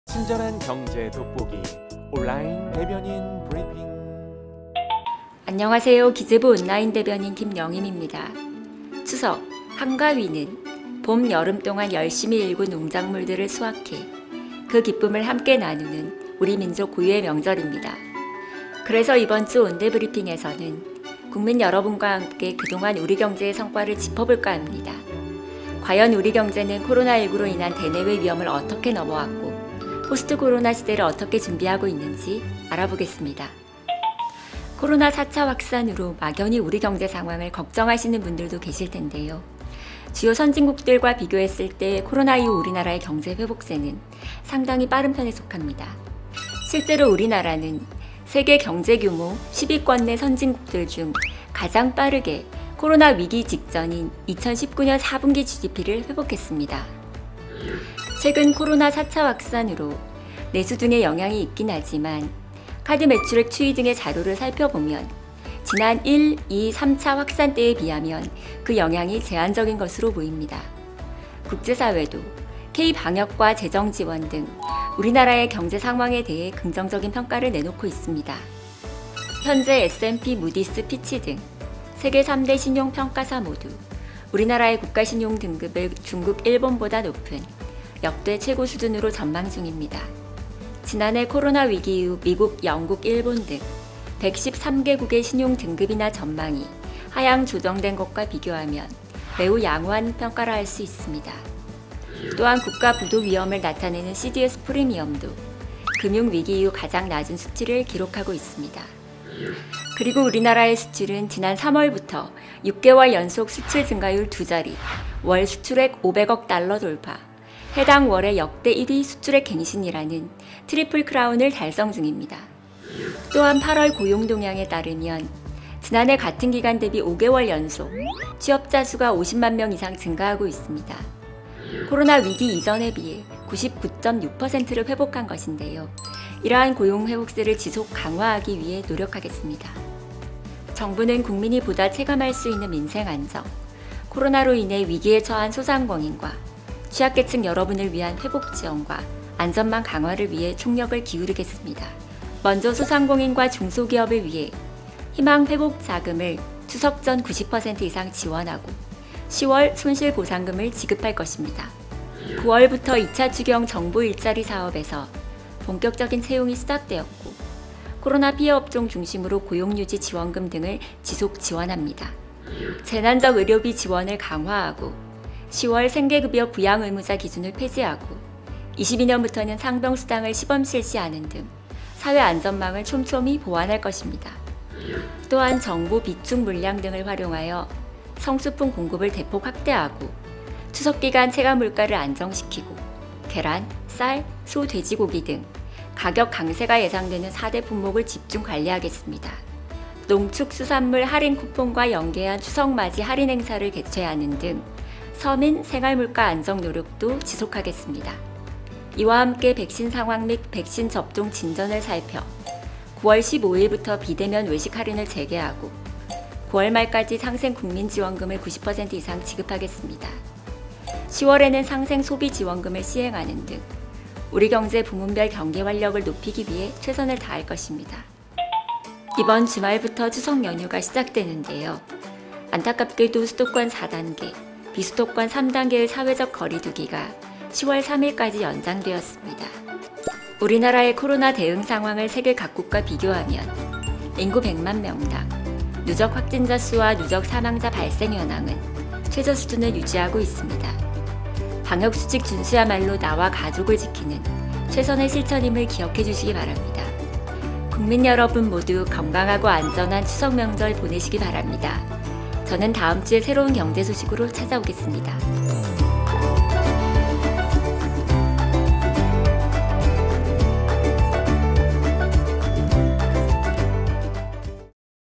🌾추석을 맞아🌾 짚어보는 우리 경제! | 온대브리핑
추석을 맞아
그동안 우리경제의 성과를 짚어봅니다!

코로나19 위기를 헤쳐나가고 있는
우리나라의 경제 상황과
대내외 평가를 살펴
포스토코로나 시대의 성장동력을 모색합니다.

✔️ [00:00] 온라인 대변인 브리핑! 시작합니다
✔️ [00:34] 우리나라 경제 진짜 괜찮나요?라는
                    생각을 갖고 있다면 클릭!
✔️ [03:49] 건강하고 안전한 추석명절 보내세요:)